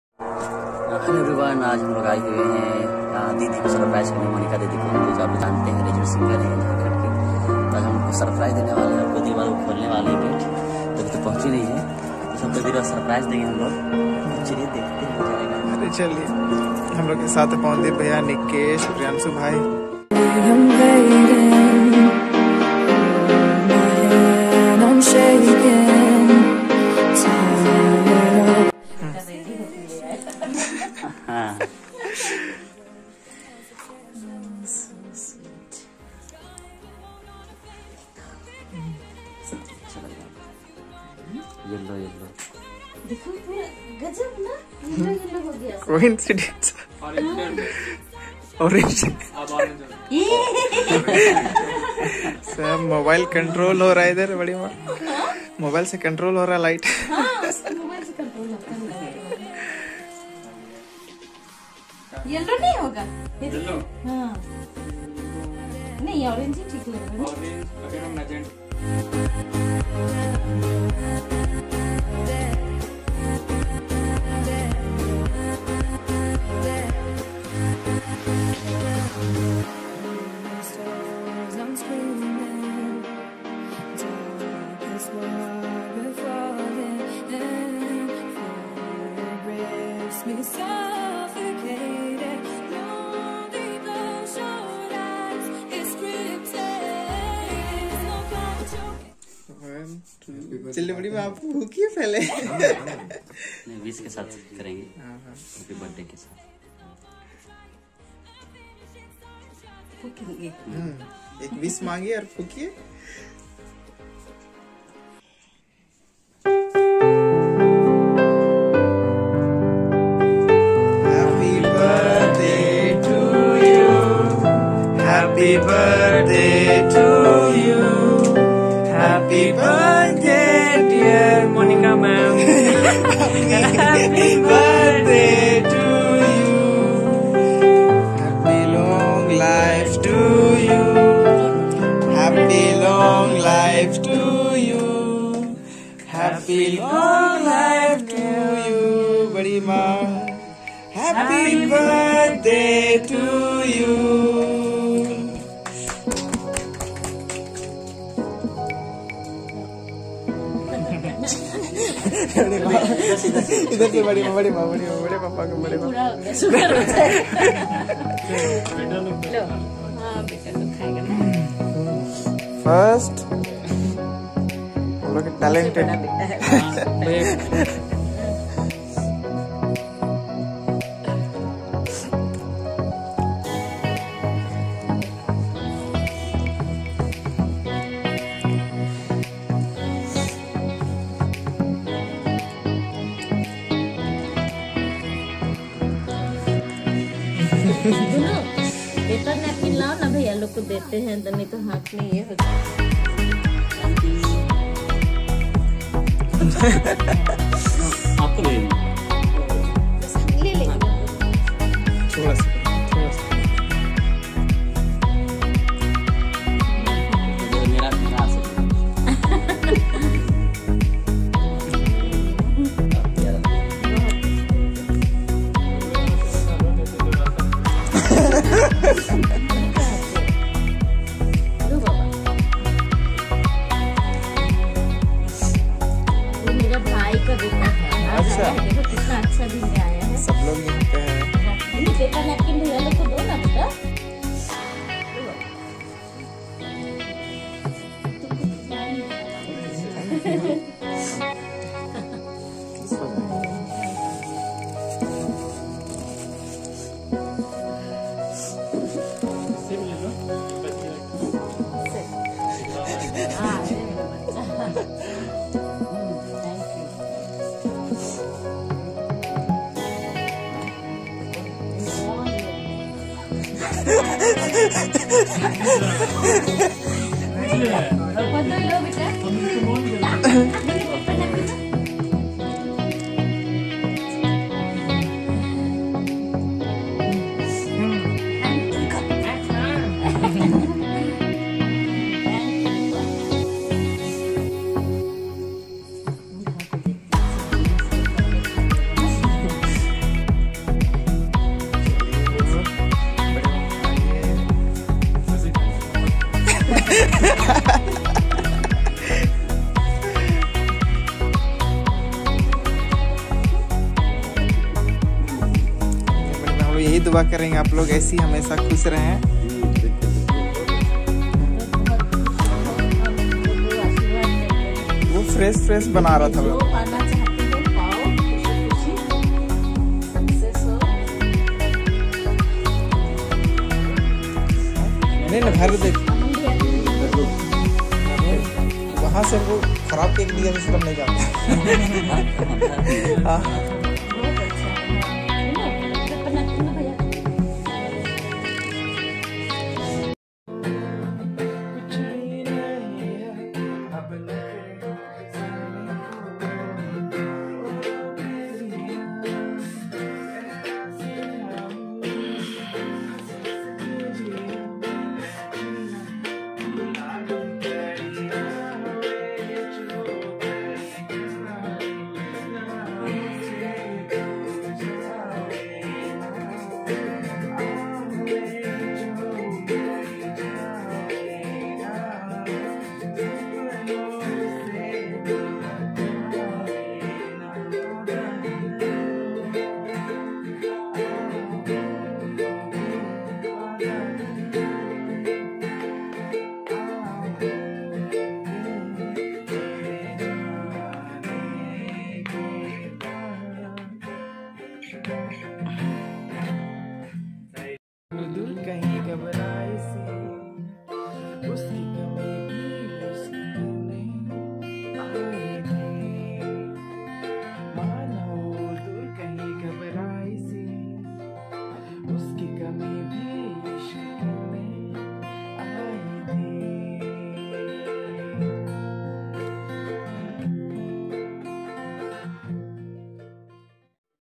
BIRTHDAY CELEBRATION //JHARKHAND LAGENT SINGER MONIKA MUNDU..//HUMLOGO NE UNHE SURPRISE DEYA...
BIRTHDAY CELEBRATION //JHARKHAND LAGENT SINGER MONIKA MUNDU..
//HUMLOGO NE UNHE SURPRISE DEYA...

BIRTHDAY-17-02
 BEST SINGER OF  JHARKHAND
SONG CREDIT-